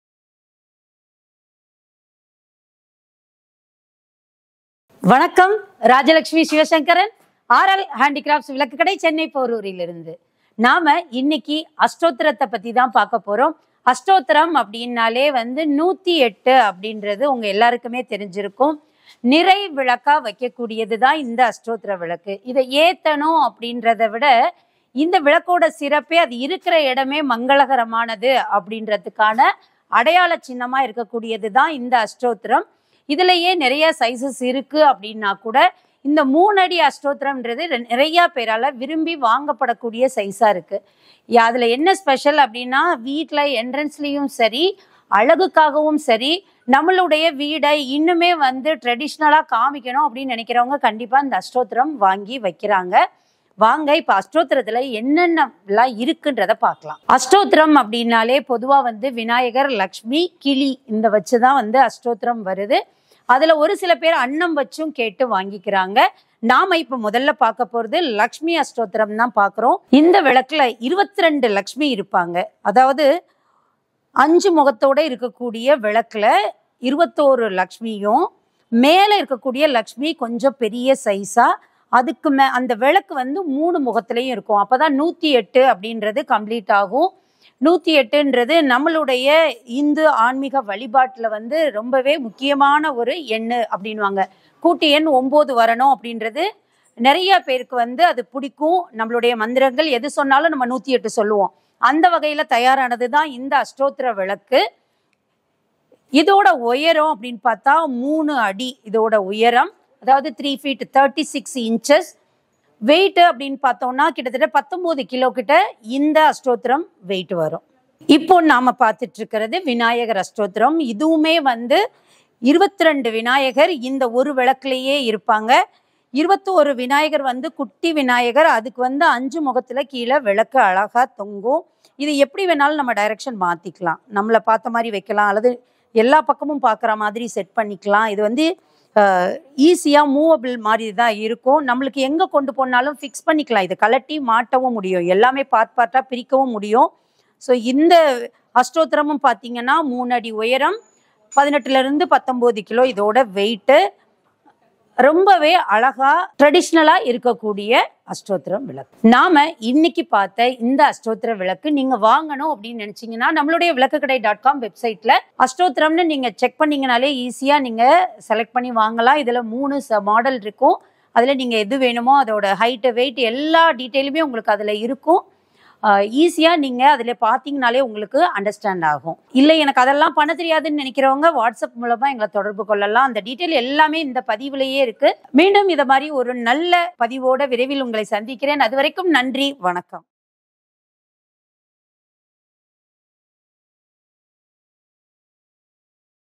மங்கள பொருளாக பார்க்கப்படும் அஷ்டோத்திர விளக்கு..! | Vilakkukadai | RLHandicrafts
நிறை விளக்காகவும், மங்கள பொருளாகவும் பார்க்கப்படும்
அஷ்டோத்திர விளக்கு..!

*Location : RL Handicrafts Vilakku Kadai
5, Ramakrishna St, Thirumurugan Nagar,
Porur, Chennai,
Tamil Nadu 600116